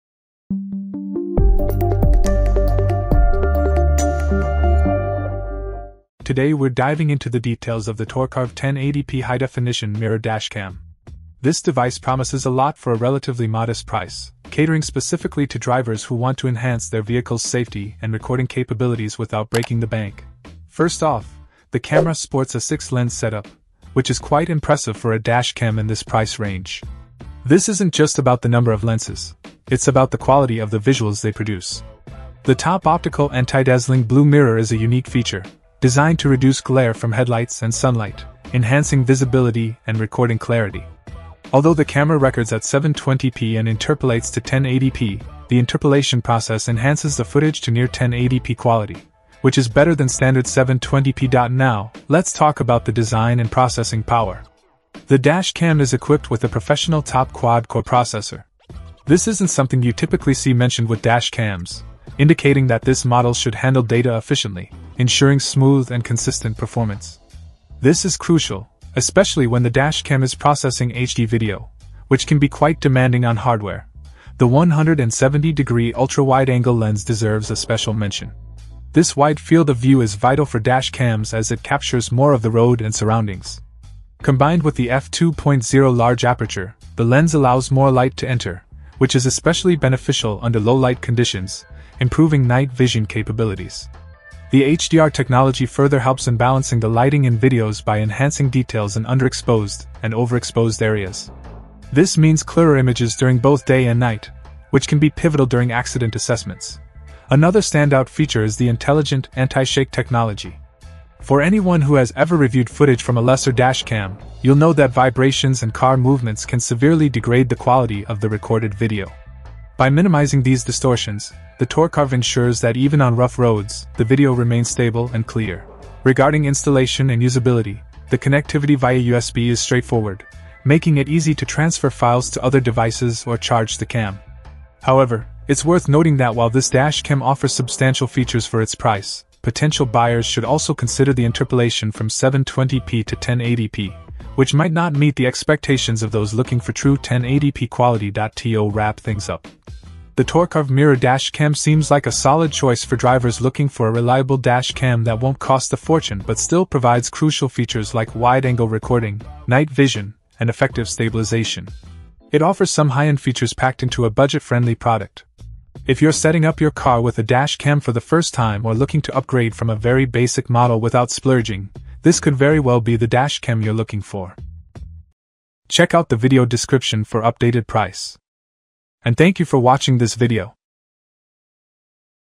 Mirror Dash Cam - Torcarvh 1080p: Best Budget-Friendly Dash Cam for Night Vision and Parking?
0:00 Intro
0:06 Review

Things that we mentioned in this video:
Mirror Dash Cam - : B0BJCTBG6H